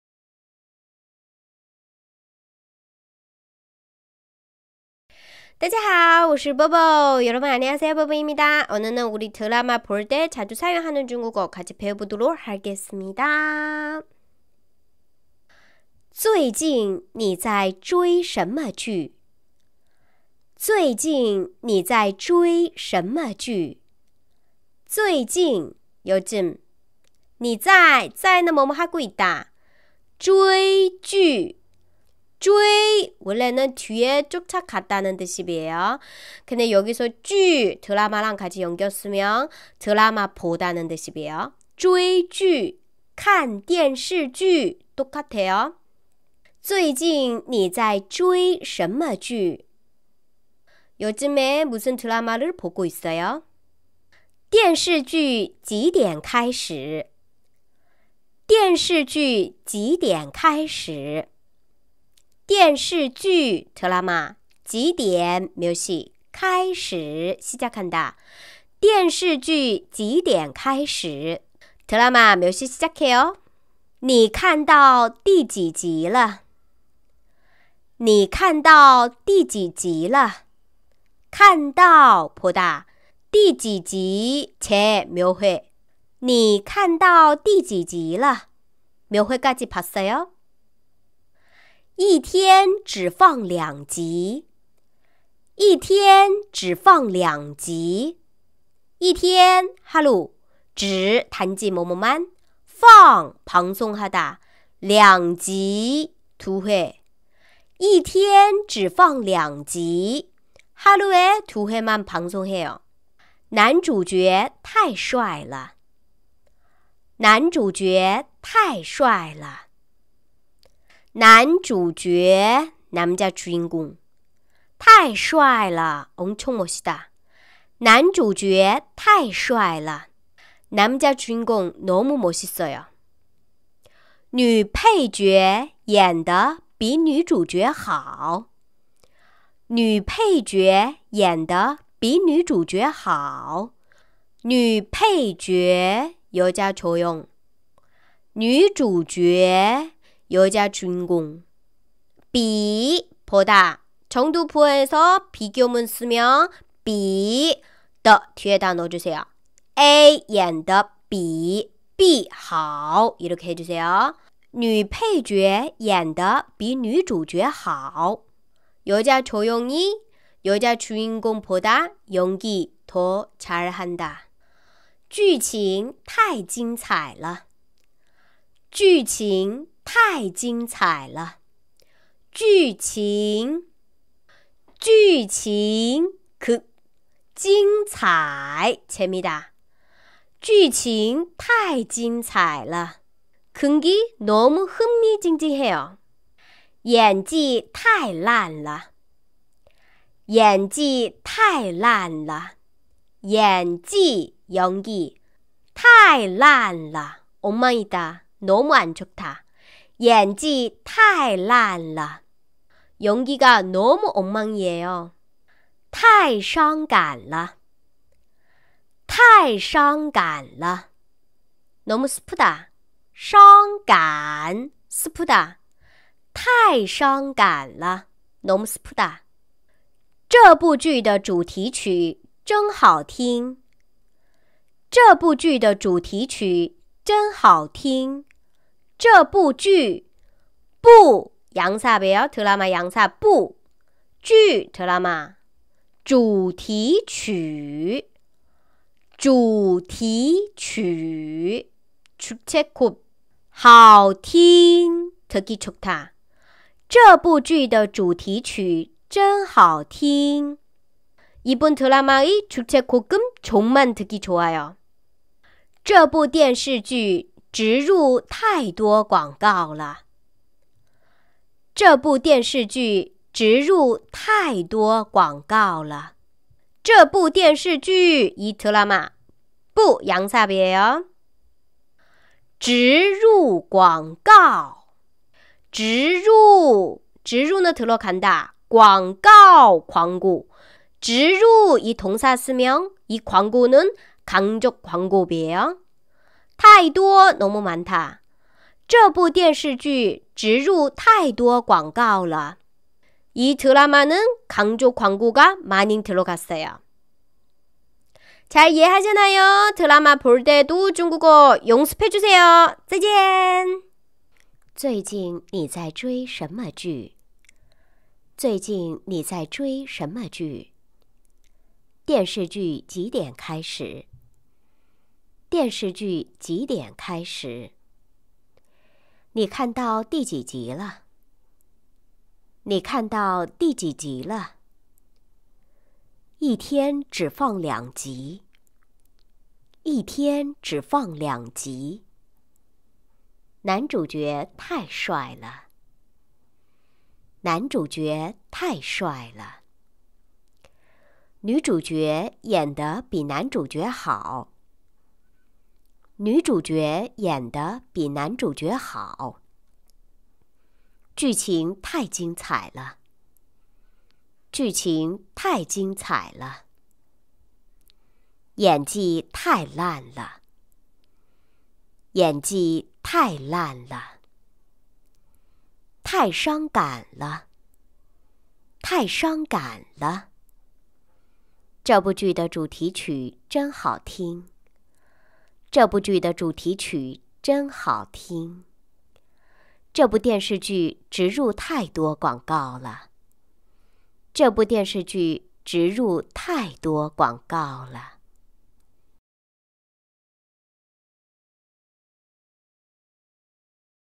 드라마 볼 때 자주 사용하는 중국어 看电视剧时常用的汉语
드라마 볼 때 자주 사용하는 중국어
1.   最近你在追什么剧？
요즘에 무슨 드라마를 보고 있어요?
2.   电视剧几点开始？
드라마 몇시 시작해요?
3.   你看到第几集了？
몇회까지 봤어요?
4.   一天只放两集
하루에 2회만  방송해요
5.   男主角太帅了
남자 주인공 너무 멋있어요
6.   女配角演得比女主角好
 여자 조연이 여주인공보다 연기를 더 잘한다.
7.   剧情太精彩了
극이 너무 흥미진진해요
8.   演技太烂了
연기가 너무 엉망이에요
9.   太伤感了
너무 슬퍼요
10.   这部剧的主题曲真好听
이번 드라마의 주제곡은 정말 듣기 좋아요
11.   这部电视剧植入太多广告了
이 드라마는 간접광고가 많이 들어갔어요